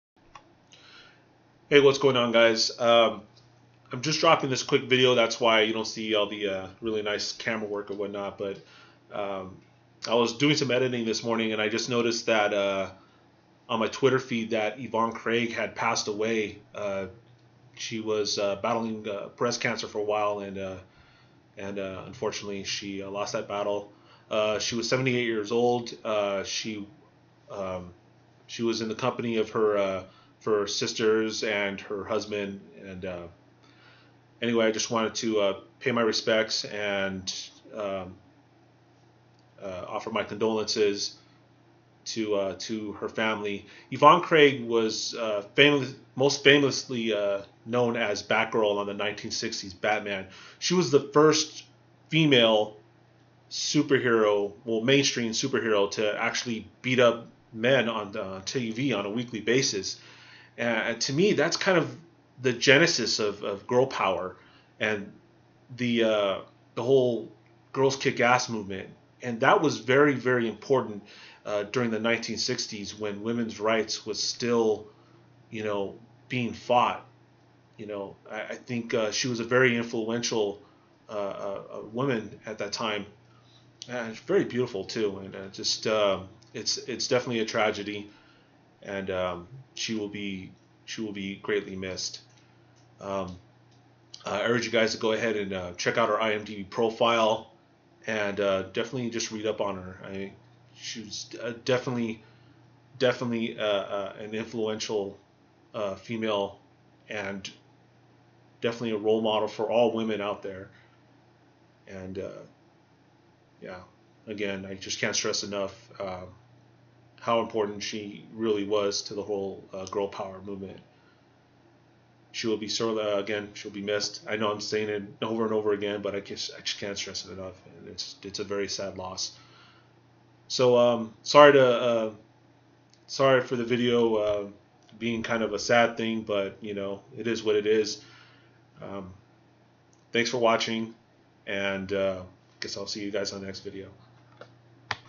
RIP Yvonne Craig AKA Batgirl Dies From Breast Cancer at 78
Just a quick video paying my respects to Yvonne Craig

GAME WITH ME ON STEAM, XBONE, AND PSN: maxsterling619